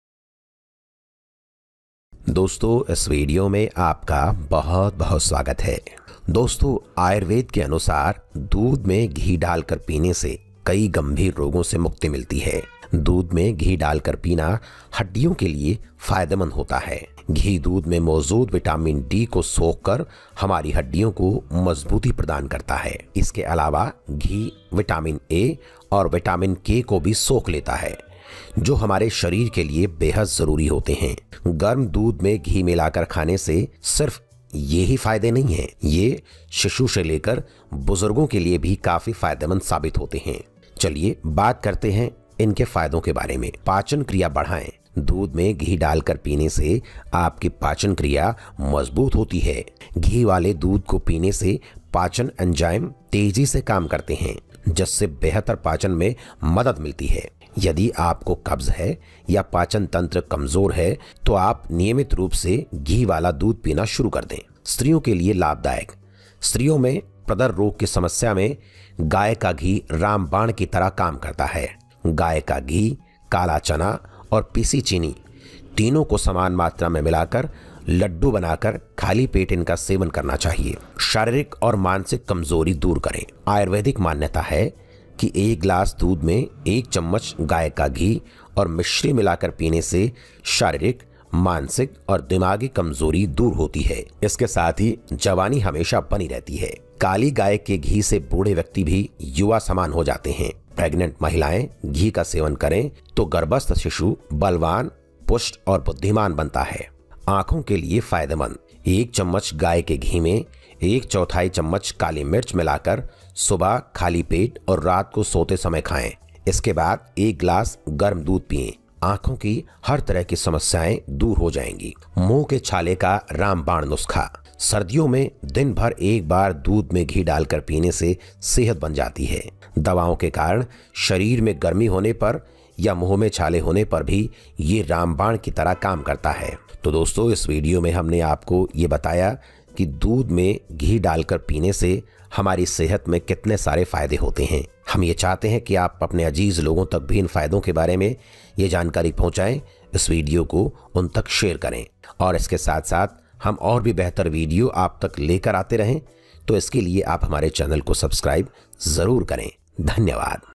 हफ्ते में 3 दिन दूध में घी डालकर पीना इससे जो होगा चौका देगा || Milk With Ghee
हफ्ते में 3 दिन दूध में घी डालकर पीना इससे जो होगा चौका देगा || Milk With Ghee ऐसी जानकारी किसी ने नहीं बताई होगी